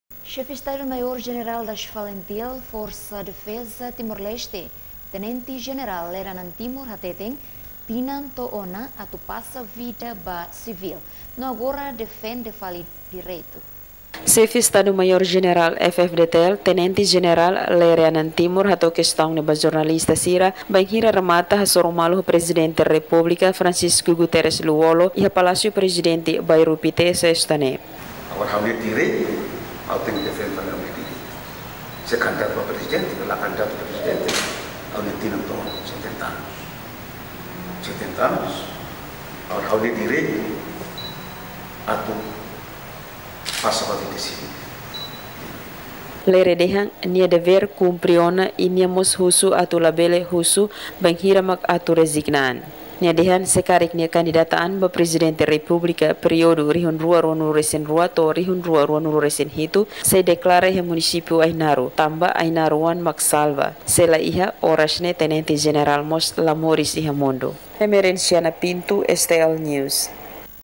Lere “Tinan Too Ona, Pasa Vida Sivil” (News 14-01-2022)
Xefe Estadu Maior Jeneral Das Falintil Forsa Defeza Timor Leste (F-FDTL), Tenente Jeneral Lere Anan Timur, hatete tinan too ona atu pasa vida ba sivil, no agora defende fali direitu.